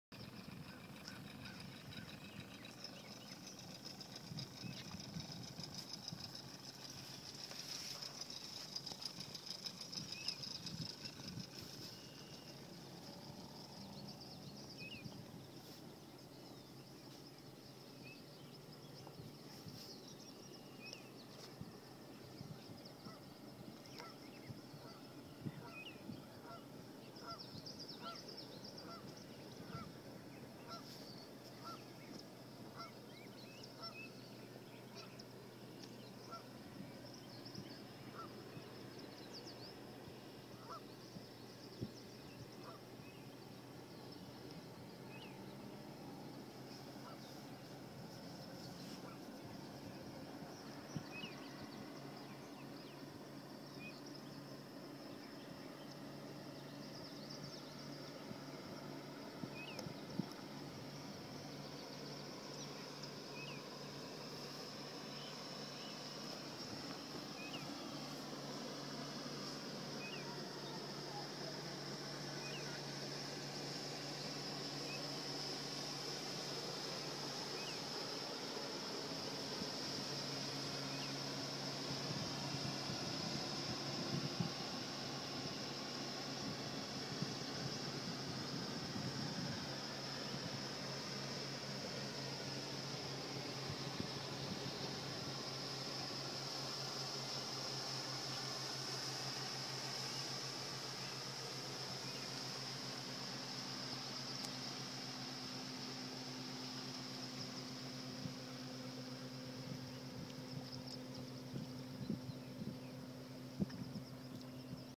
Gar pike spawning
Just about 50 or more gar pike